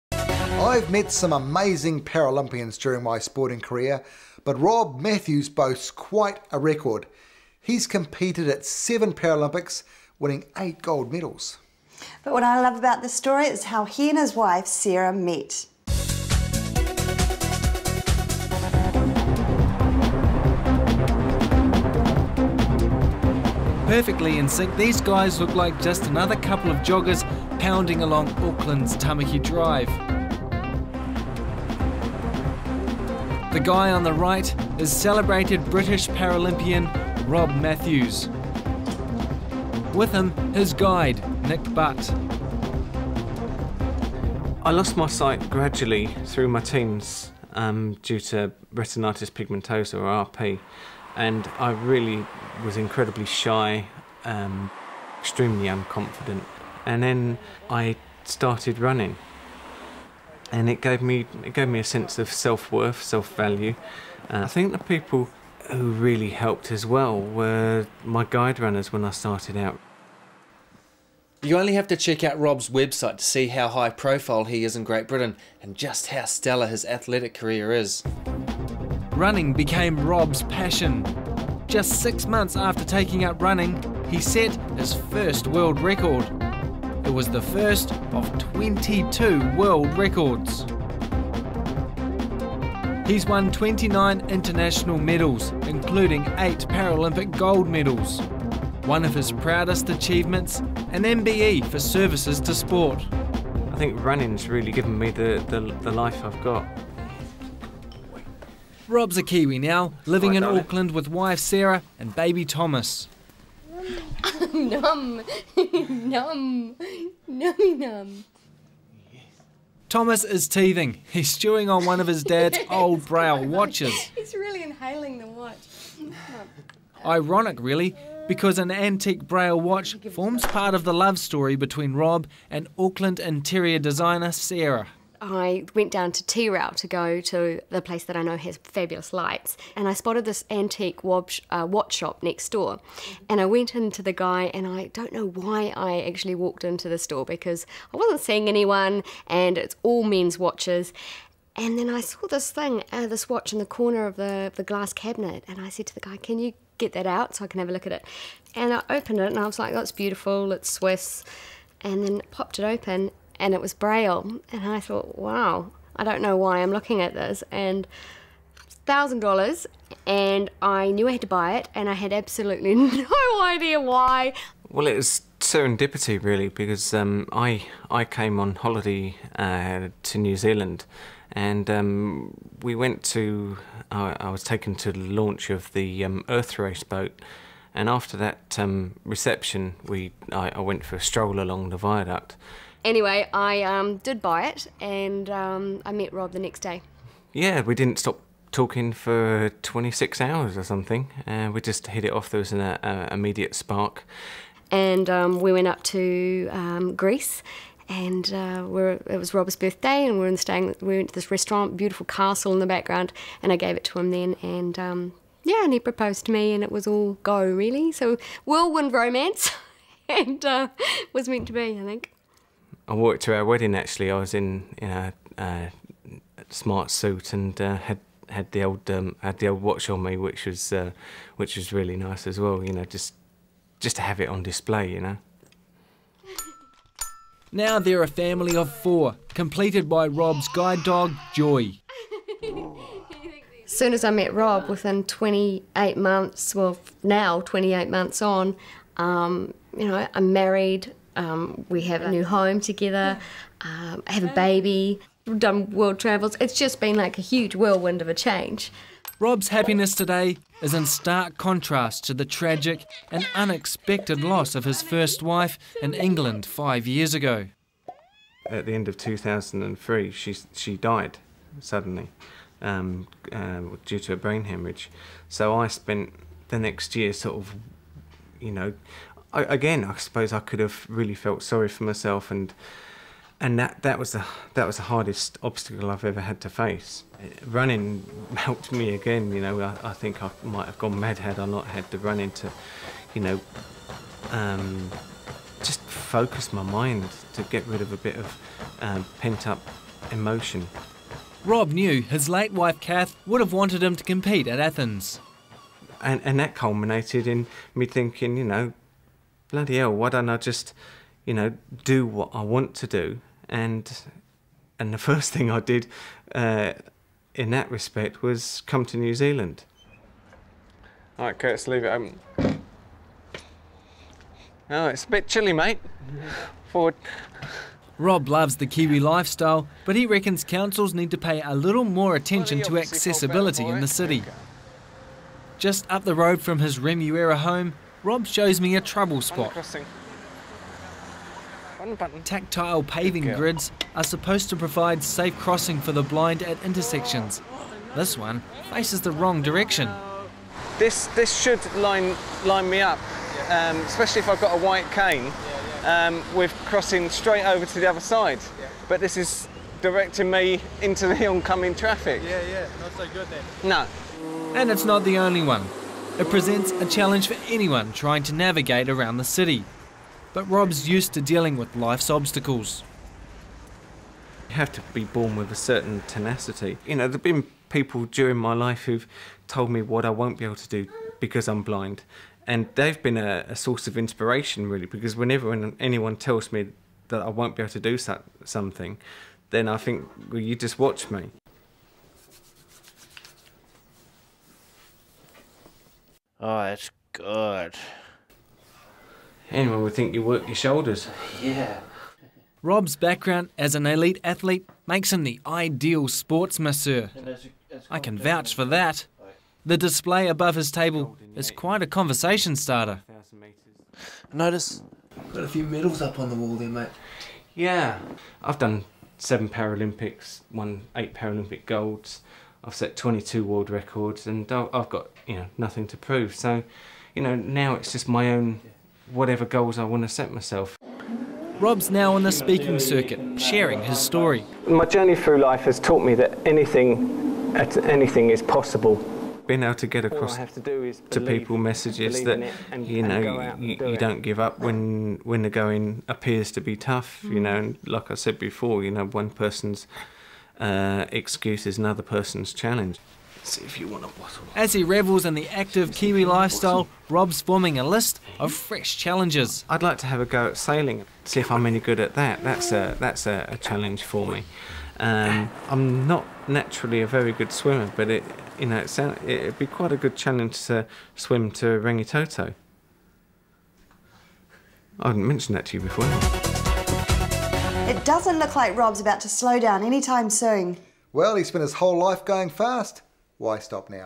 Rob Matthews
British Paralympic legend Rob Matthews tells us why he chose NZ to start a new life, following the tragic loss of his wife.